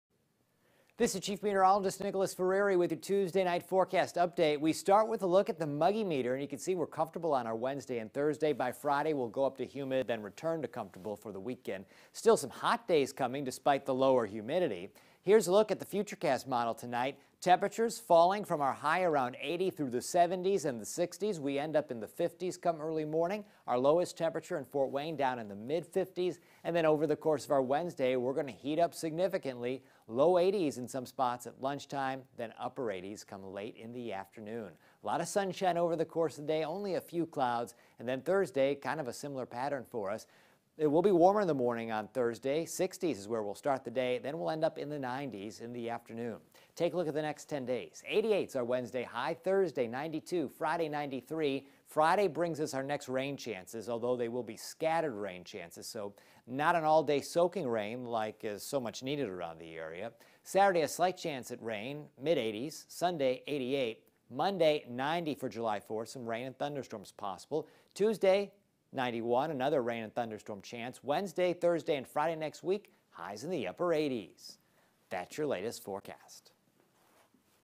Tuesday Night Forecast Update
Temperatures fall to the mid-50s by dawn.